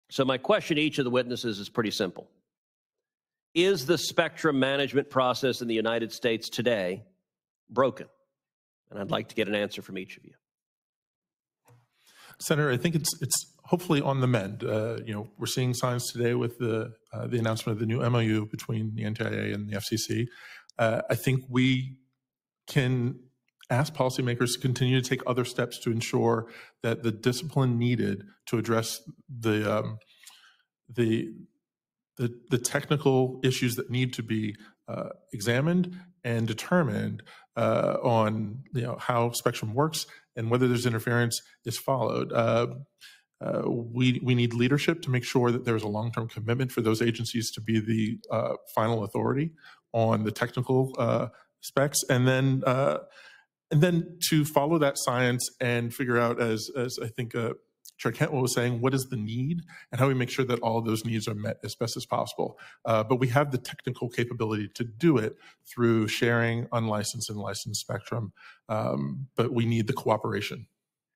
Senate Commerce Subcommittee Spectrum Hearing: Sen. Cruz
Questioning from Sen. Cruz during an August 2022 hearing on the future of spectrum. He asks if our spectrum management process is broken.